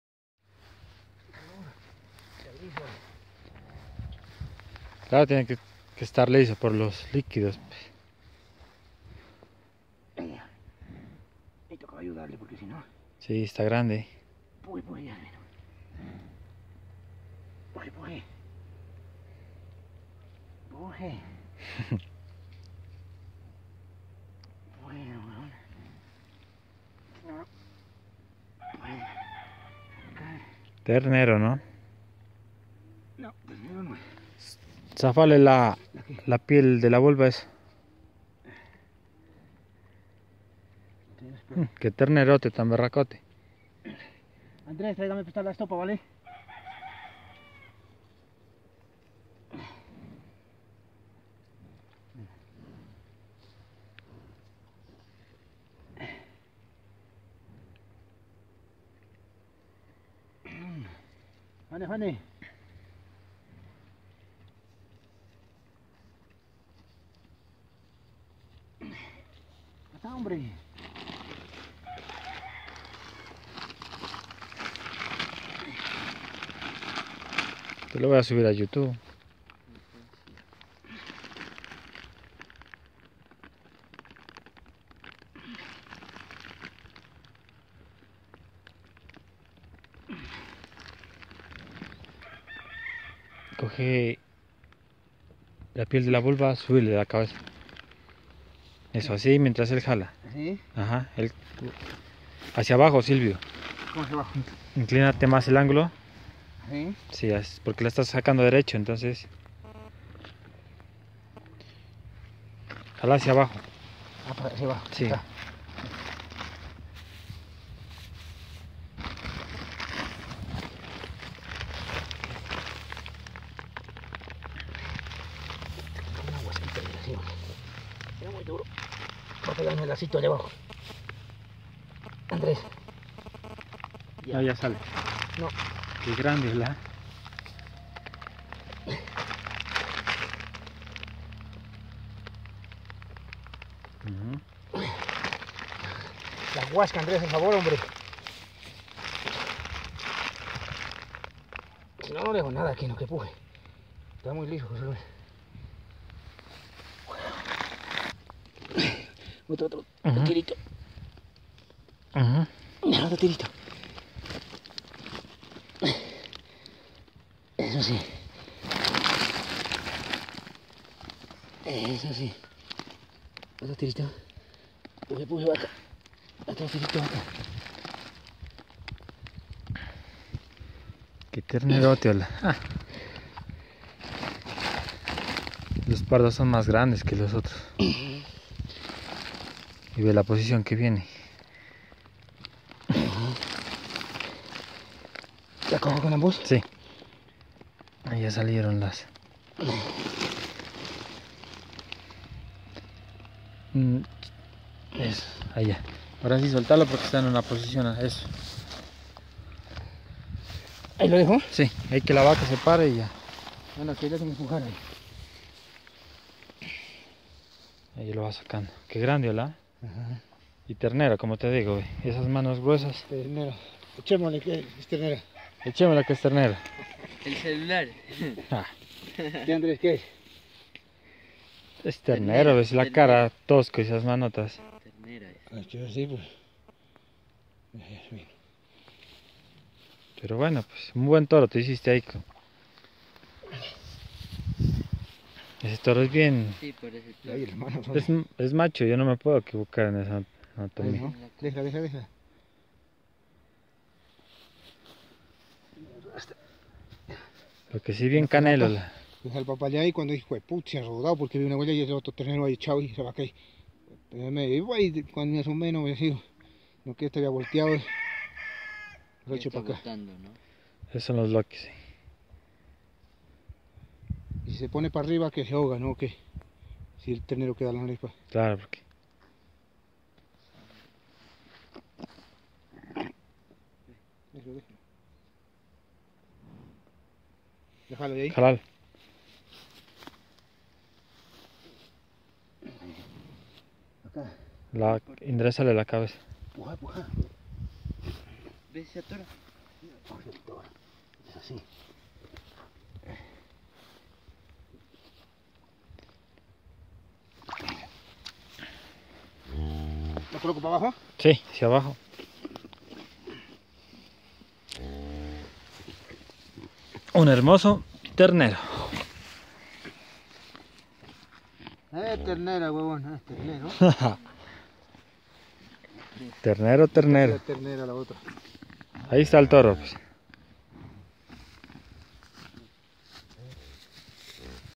Parto de vaca (ternero 3/4 Pardo Suizo Americano). Ocampo en acción.
Vereda los Frailes. Parto de ternero 3/4 pardo suizo americano, obtenido mediante la tecnica de inseminacion artificial.
Extracción de una forma cochinita por parte del dueño de los animales. Pero que al final no causó problemas en la salud humana y animal (Recomendación: utilizar guantes de palpación más de cirugia).